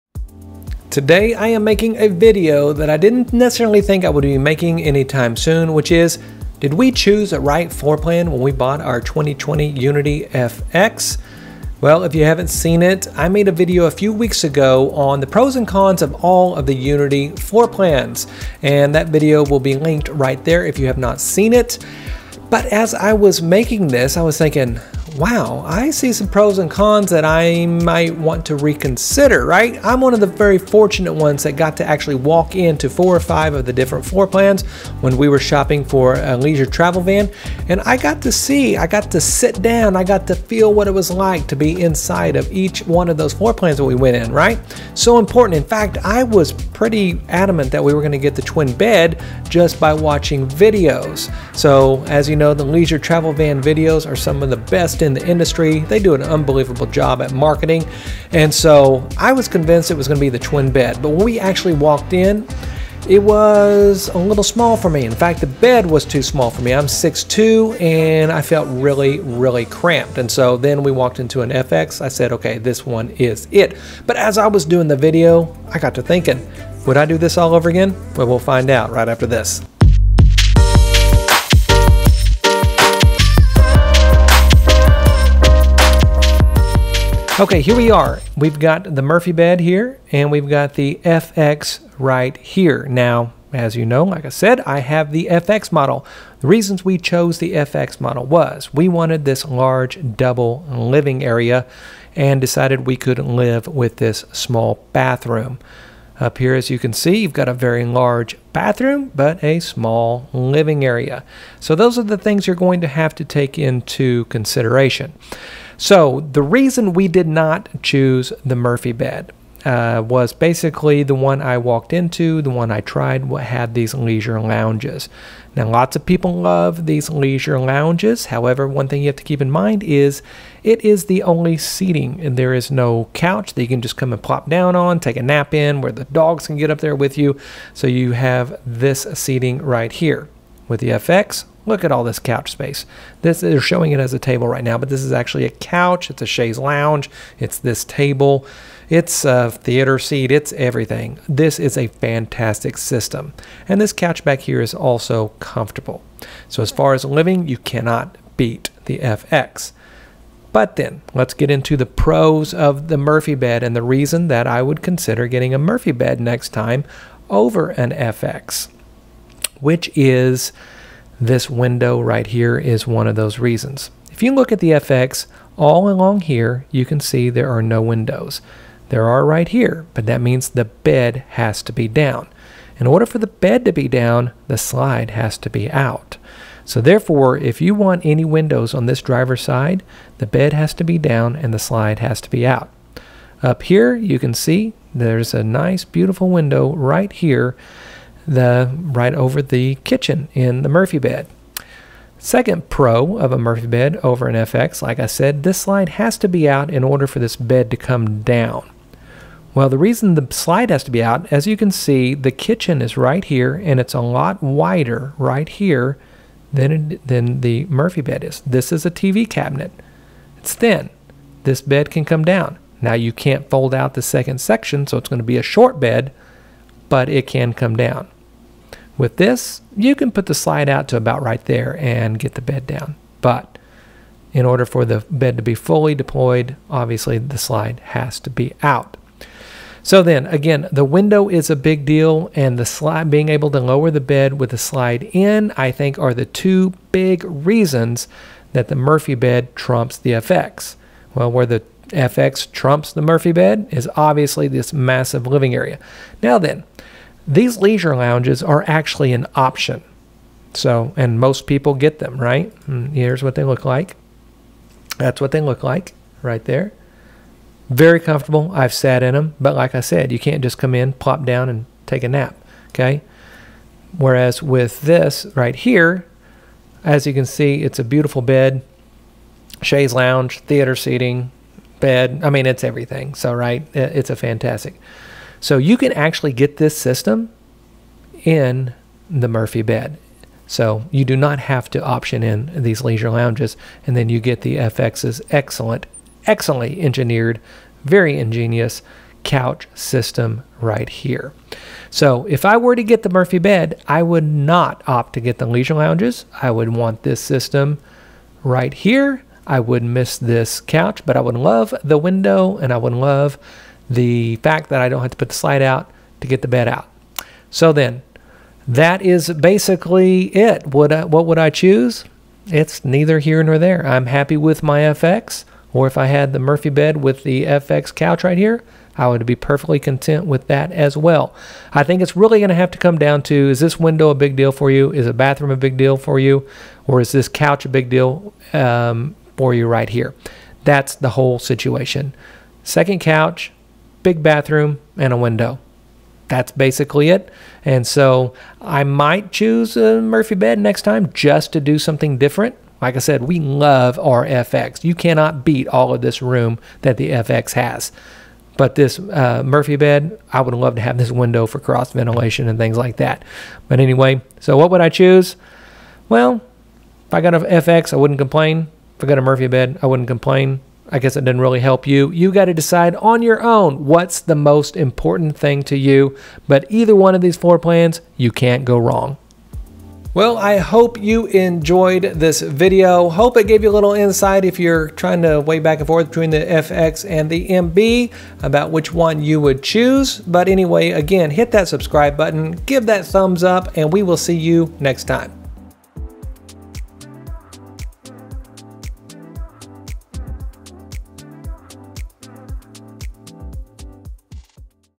Did we choose the right floor plan in our Leisure Travel Van, would I choose the FX again?
After I uploaded my video on the pros and cons of the Unity floorplan, it made me ask myself, would I choose the FX again in my leisuretravelvan

Here I discuss the other option I would choose, the MB and the pros and cons versus the FX. LTV unity wonder @Leisure Travel Vans

Start: 0:00
Floor plan compare: 1:29
Summary: 6:18